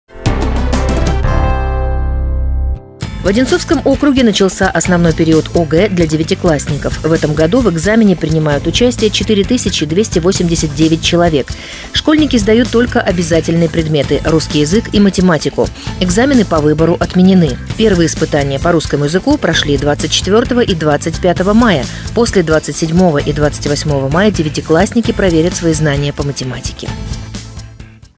ОГЭ-2021
В Одинцовском округе начался основной период ОГЭ для девятиклассников. В этом году в экзамене принимают участие 4289 человек. Школьники сдают только обязательные предметы —  русский язык и математику.